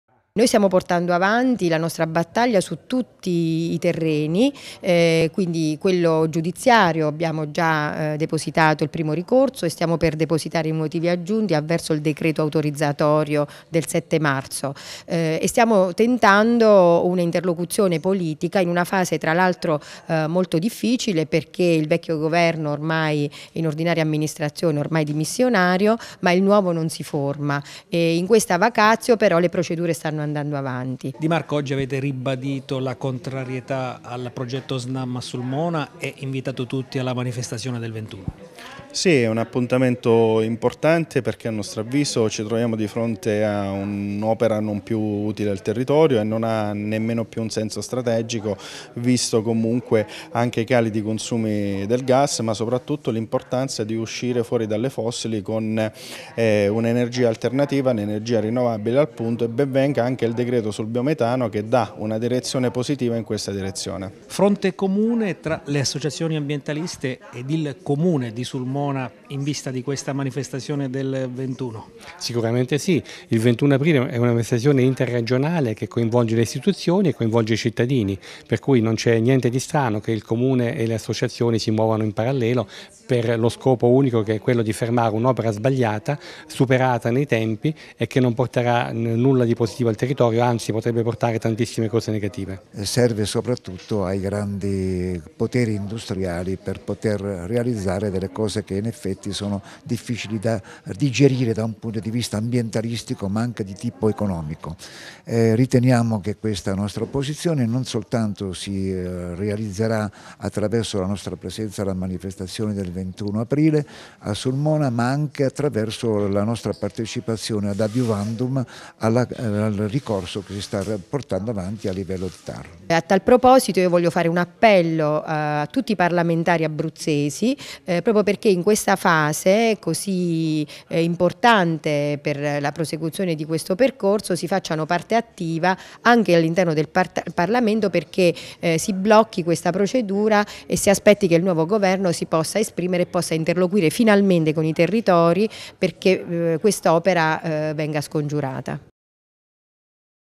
WWF e Legambiente contro la Centrale Snam di Sulmona
WWF e Legambiente continuano la loro battaglia, anche legale, contro la centrale di compressione SNAM di Sulmona. Le associazioni ambientaliste parteciperanno alla mobilitazione del 21 aprile per ribadire il NO di cittadini, istituzioni e associazioni a un progetto che definiscono dannoso e sostanzialmente inutile. Intanto, questa mattina, sono state esposte le ragioni del ricorso al TAR